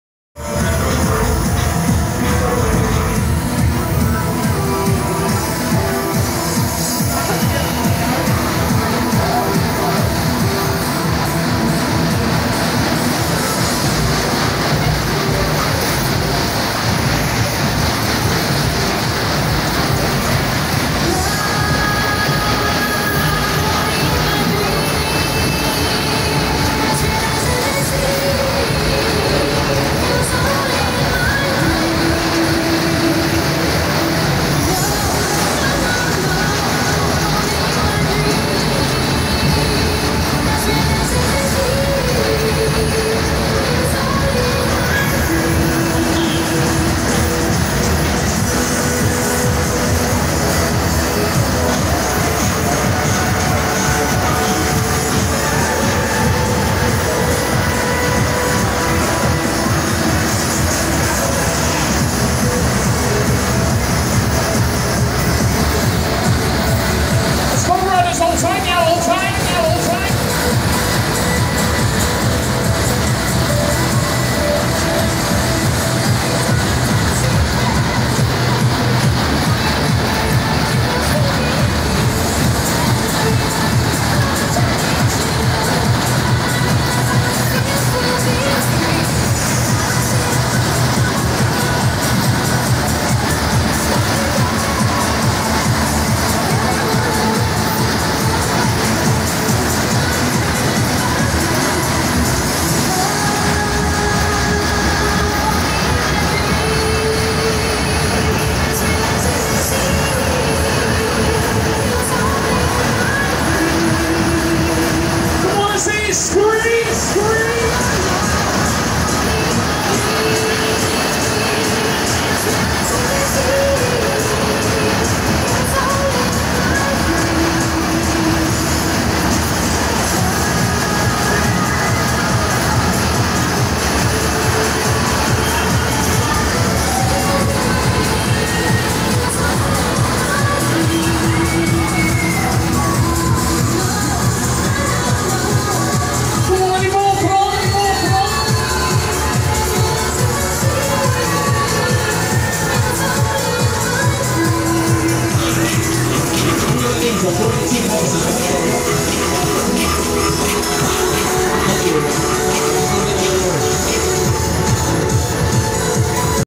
Ryan Pearson's Midnight Express (Sobema Matterhorn) offride
Video taken at Boston May Fair on the 4th May 2024. A video that I did for @Fairgroundking1. :)